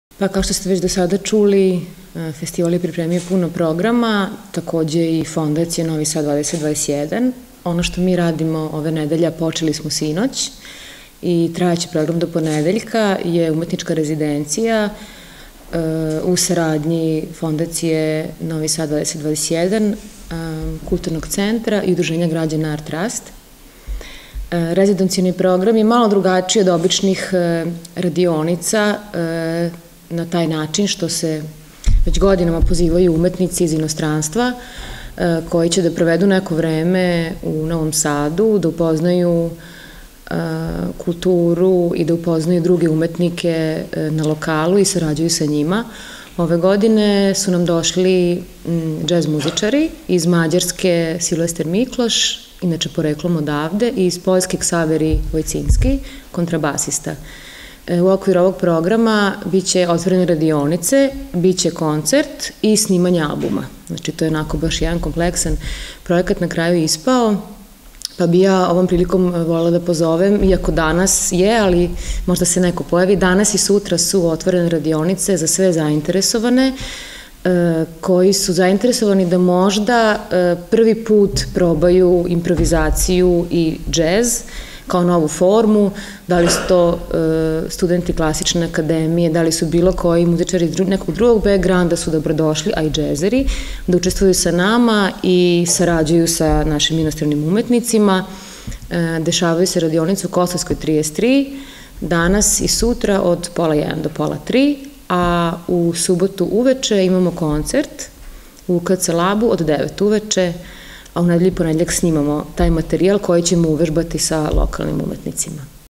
Јасна Јовићевић, руководилац Интернационалне музичке резиденције у оквиру фестивала
Руководилац Интернационалне музичке резиденције у оквиру фестивала, Јасна Јовићевић, рекла је да се резиденционални програм разликује од радионица. Џез музичари из иностранства провешће неко време у Новом Саду, упознати локалне уметнике и успоставити међусобну сарадњу. Ове године дошли су Силвестер Миклош (Мађарска) и Ксавери Лицински из Пољске.